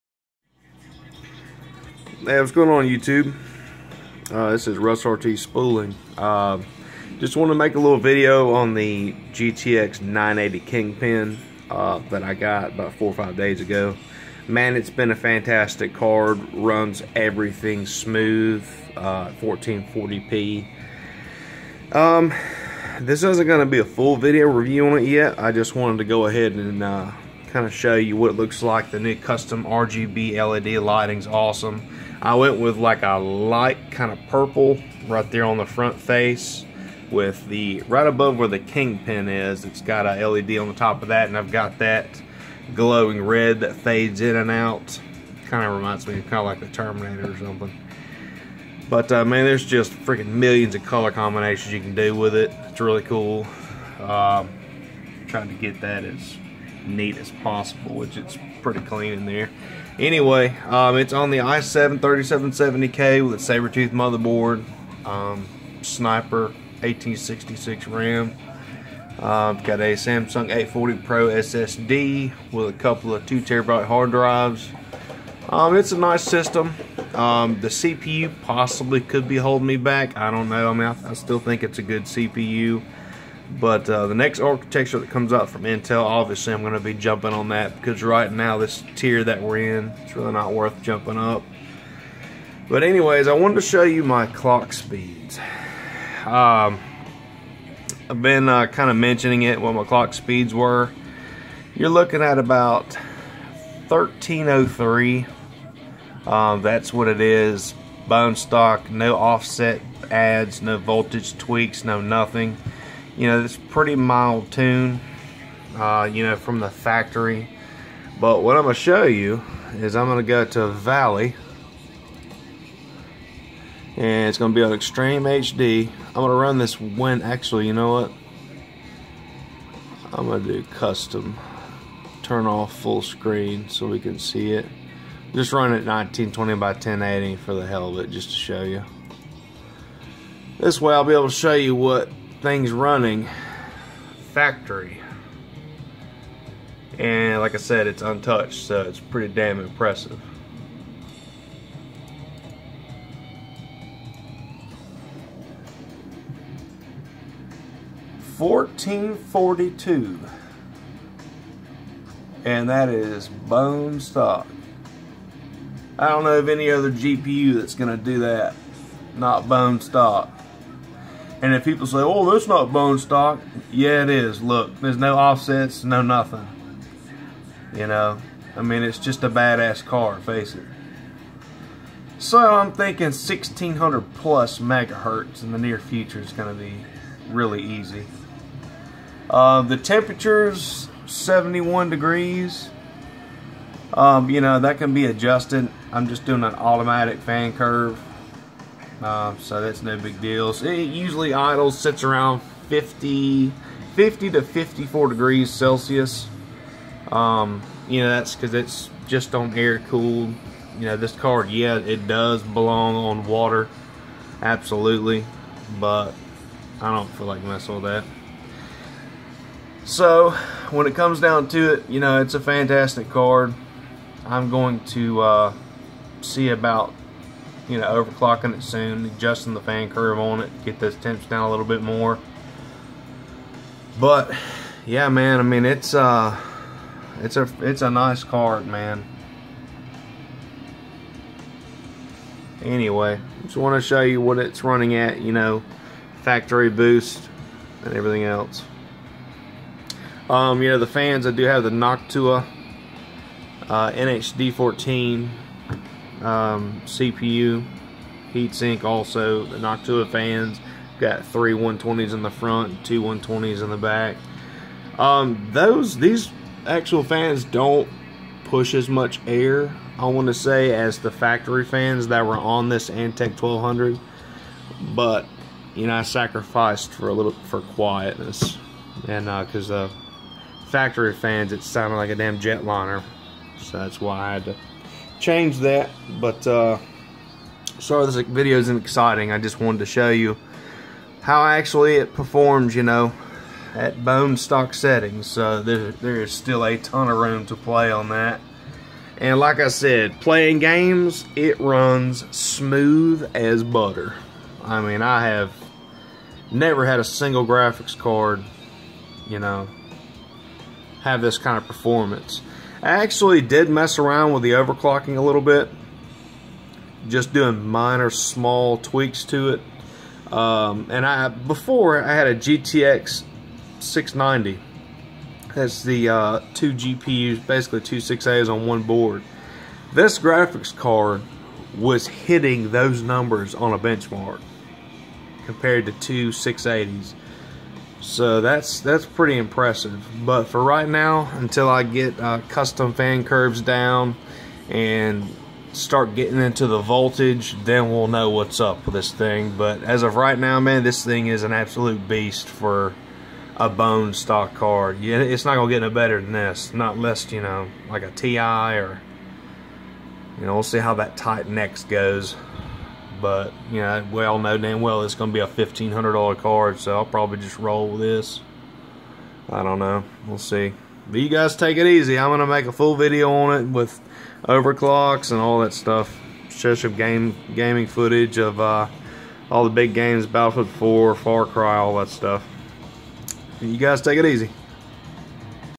EVGA GTX 980 Kingpin factory clock speeds
This is a little video of my 980 kingpin and her factory clock speeds that blow away any other factory clocks on any 980.